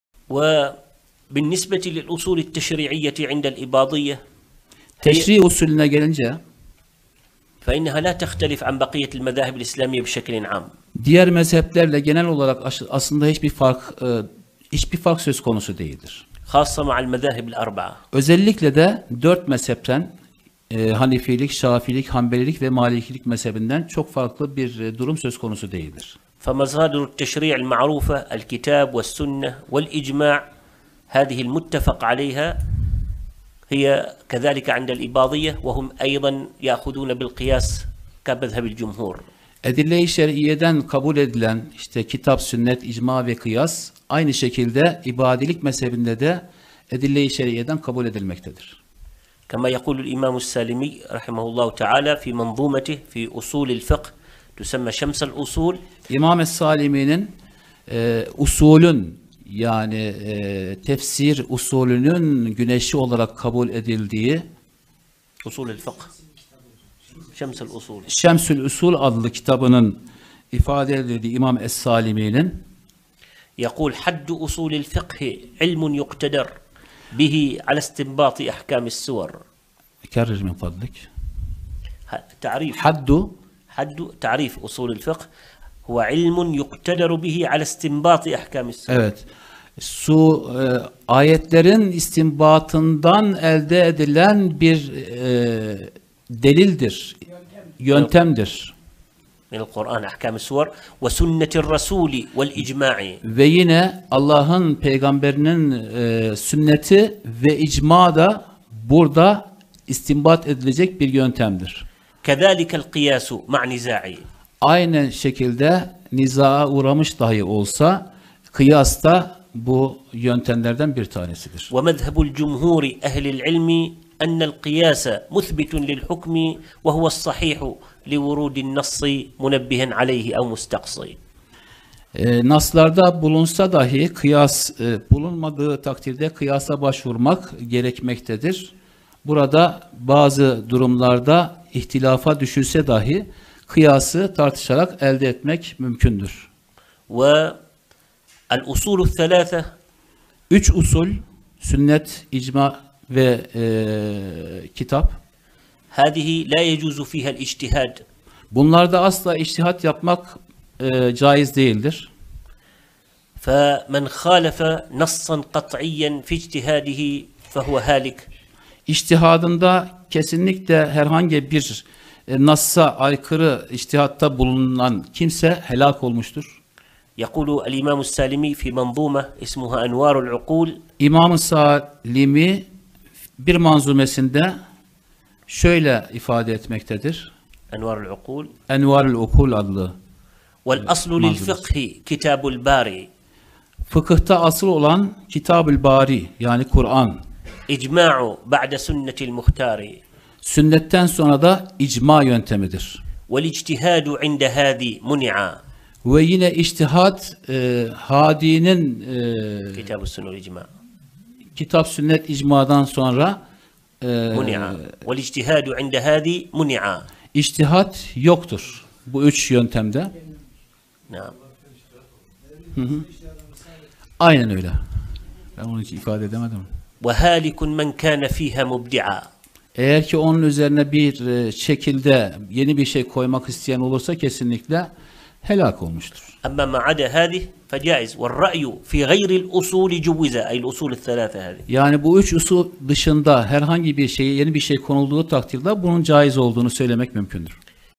الاصول والمصادر التشريعية للمذهب الاباضي. للدكتور الشيخ عبدالله المعمري
مصدر المقطع:

محاضرة مترجمة الاباضية في العصر الحاضر

للدكتور الشيخ عبدالله المعمري